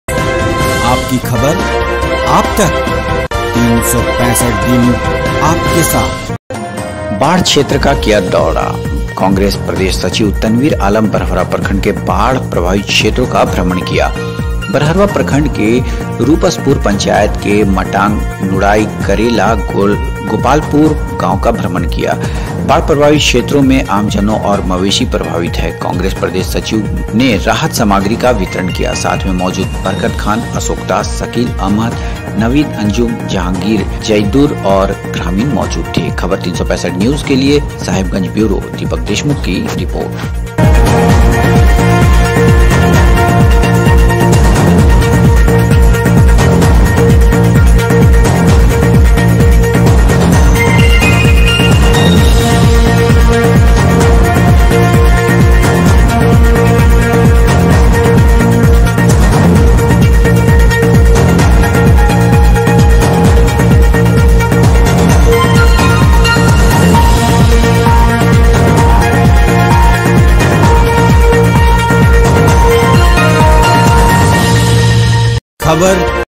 बरहरवा - बाढ़ क्षेत्र का किया दौरा
बरहरवा :- कांग्रेस प्रदेश सचिव तनवीर आलम बरहरवा प्रखण्ड के बाढ़ प्रभावित क्षेत्रों का भ्रमण किया।
बरहरवा प्रखण्ड के रूपसपुर पंचायत के मटांग, नुड़ाई, करेला, गोपालपुर गांव का भ्रमण किया, बढ़ प्रभावित क्षेत्रों में आमजनों और माबेसी प्रभावित है। कोंग्रेस प्रदेश सचिव ने राहत सामग्री का वितरण किया। साथ मे मौजूद बरकत खान,अशोक दास, सकील अहमद,नबीद अंजुम,जहांगीर,जोईदुर और ग्रामीण मौजूद। साहिबगंज ब्यूरो चीफ दीपक देशमुख की रिपोर्ट